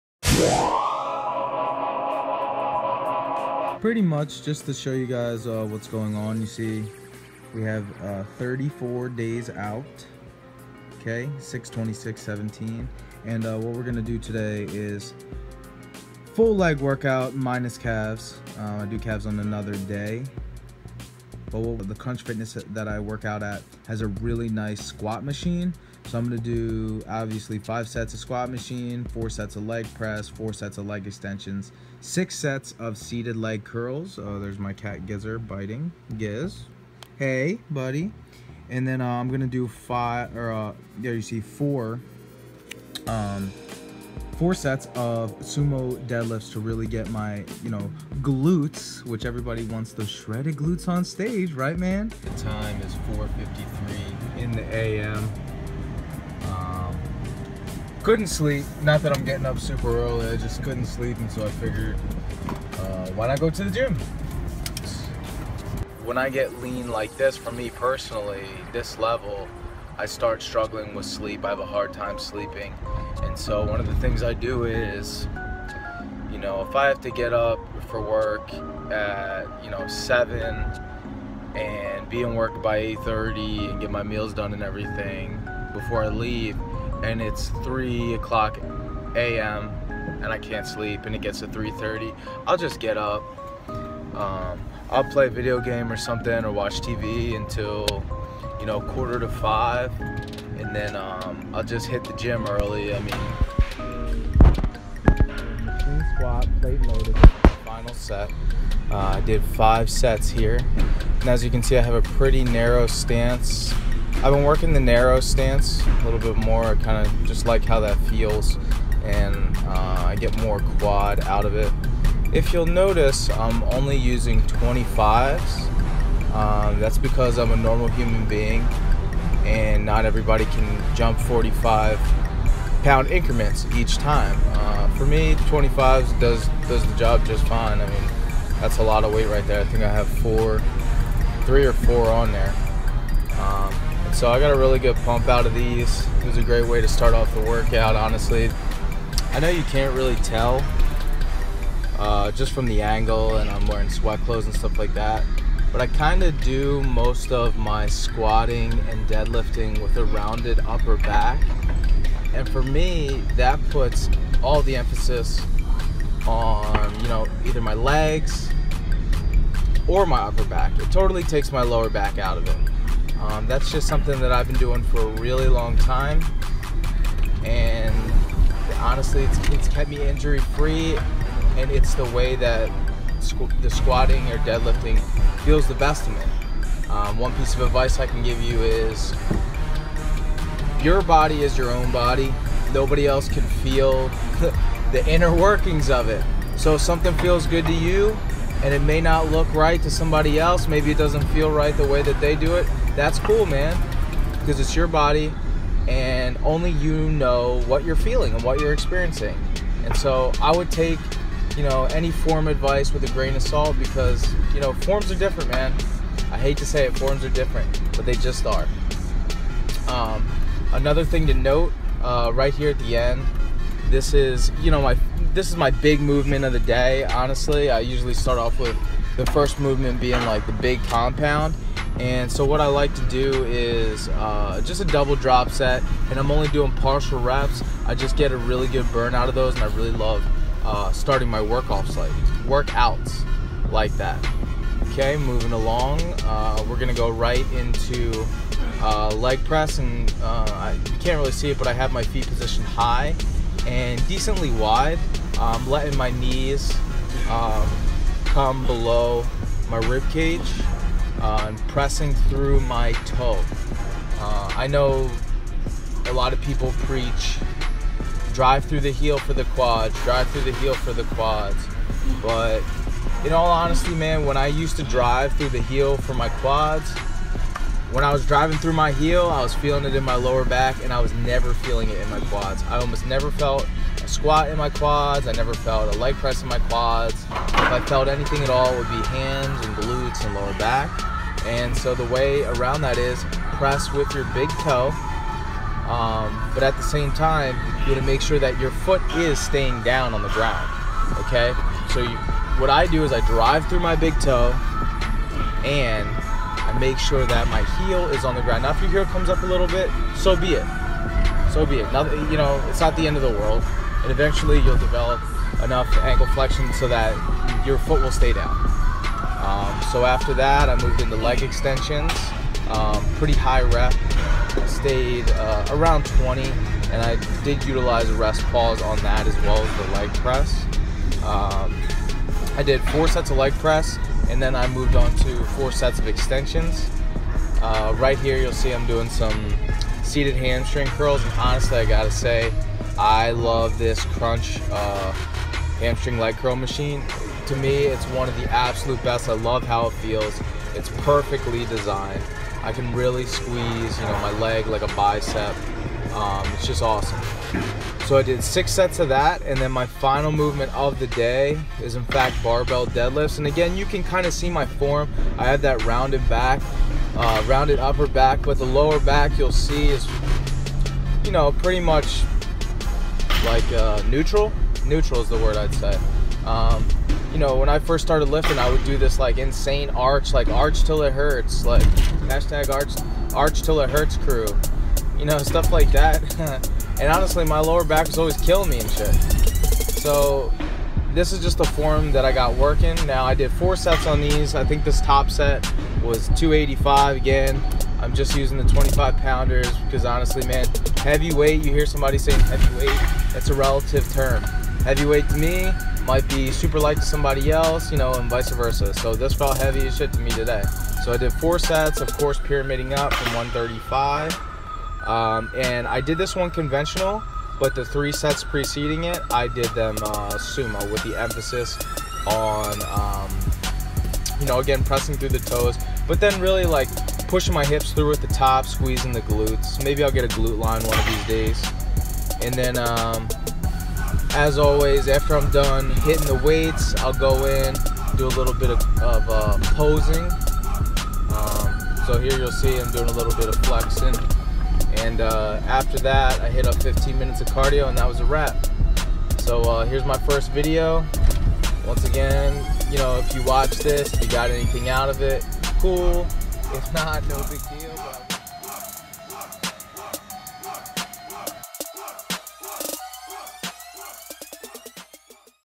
The Return of Tyjaws: 32 Days Out Legs
34 Days Out from the 2017 OCB Harklan. Short and Sweet Leg Routine posted below:
Quads:
-squat machine 5x
-leg press 4x
-leg extensions 4x
Hamstrings
-seated leg curl 6x
-deadlift 4x